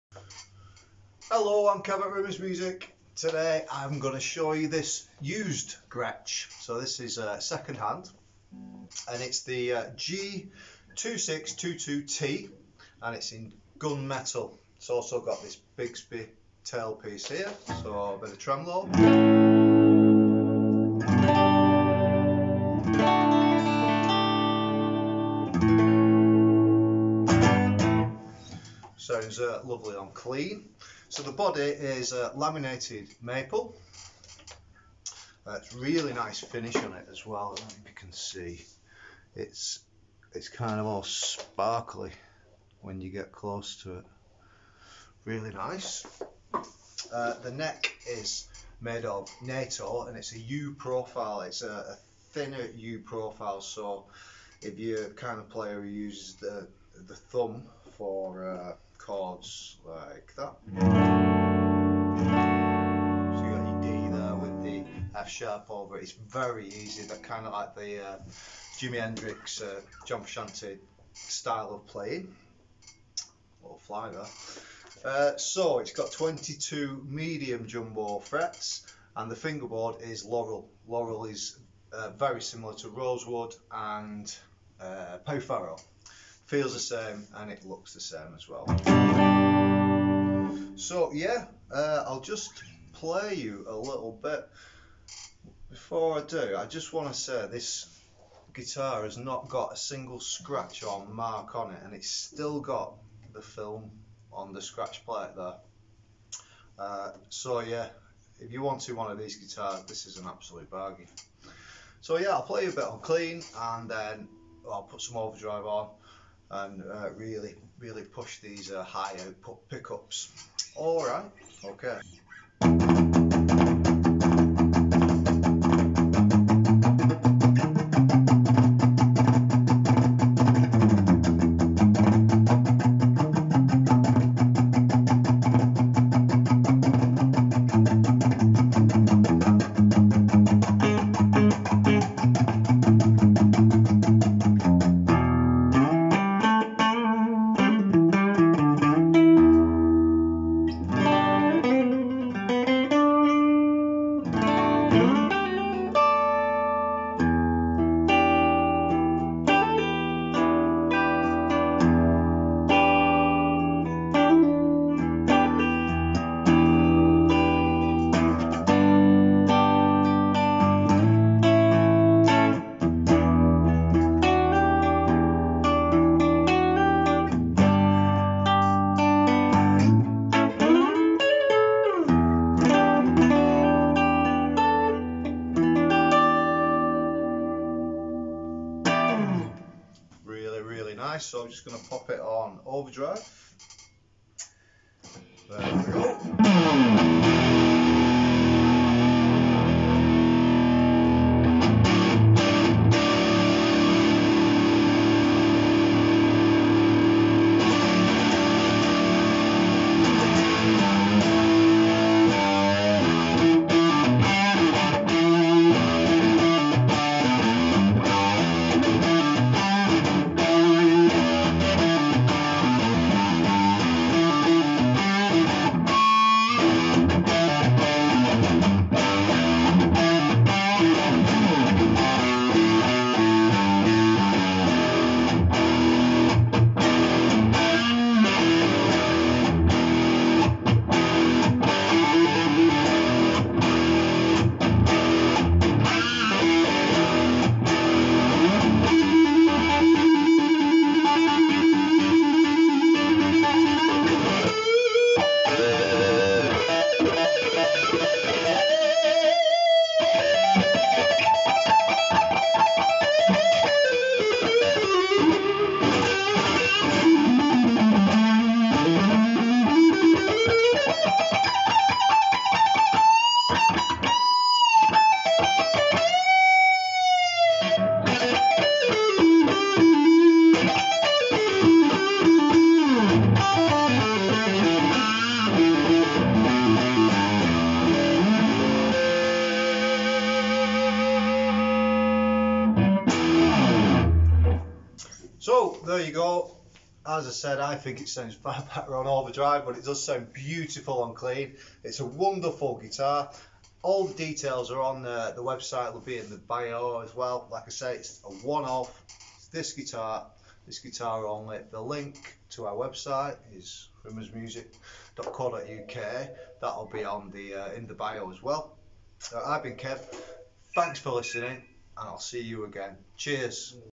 'Used' Gretsch G2622T GNMTL w/Bigsby - Rimmers Music
Here we have Kevin showing you this great used Gretsch G2622T GNMTL w/Bigsby.

The Gretsch G2622T Streamliner Center Block with Bigsby is an affordable semi-hollow guitar with some serious rock potential! With its high-output Broad’Tron BT-2S humbuckers and centre block, semi-hollow construction the G2622T Streamliner is packed full of versatility and playability!
This is the double cut version of Gretsch’s “Center Block” body, which is a laminated maple hollow body with a chambered spruce centre block which cuts down on unwanted feedback whilst maintaining some of the tonal qualities that hollow bodies provide. With a centre block guitar you can benefit from the warmer, more resonant tone provided by the hollow sections but still be able to turn up the gain and rock out!
The neck on the guitar is comfortable to play thanks to a few things. The thin “U” profile neck has rounded shoulders which is great for playing chords and wrapping your thumb around the neck, but the thinner profile makes it less cumbersome than a traditional “U” shape neck. This thinner profile, in combination with the 12” radius fingerboard also makes single note runs a breeze.
This guitar is loaded with Gretsch’s Broad’Tron BT-2S humbuckers, designed specifically for the Streamliner Collection. These pickups are high output with good low, mid, and high-end response with plenty of clarity in the highs that balances well with the natural warmth of the body. The Broad’Tron BT-2S pickups are well suited for classic country sounds all the way to punchy rock tones.
The Bigsby vibrato tailpiece is another classic Gretsch feature on this guitar. Not only does it look cool, the Bigsby allows you to throw some subtle pitch bending into your playing.

“This guitar has barely been used and is in perfect condition, without a single scratch or mark. The film is still on the scratchplate and pick ups.” Kev Atherton - RIMMERS MUSIC